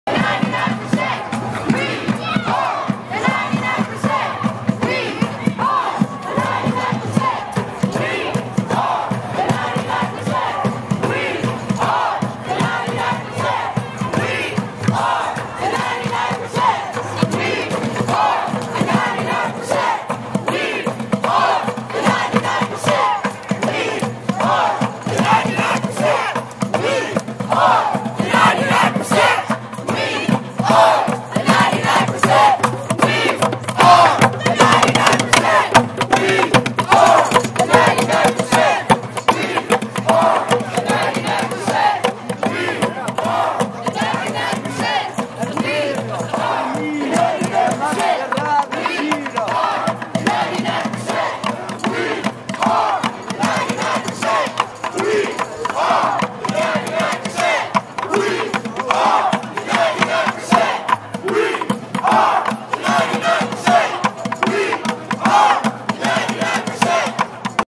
We Are the 99% [10/29/11]
"We Are the 99%" chant following the delivery of the eviction notice at the Illinois State Capitol on 10/29/11.